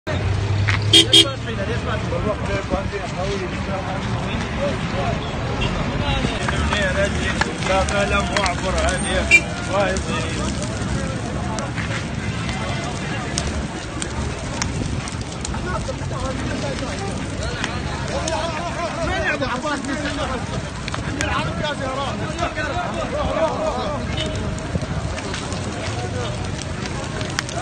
استمرار قطع الطرق الرئيسة في الناصرية من قبل المتظاهرين احتجاجًا على التسويف الحكومي لمطالب الثورة
ـــــــــــــــــــــــــــــــ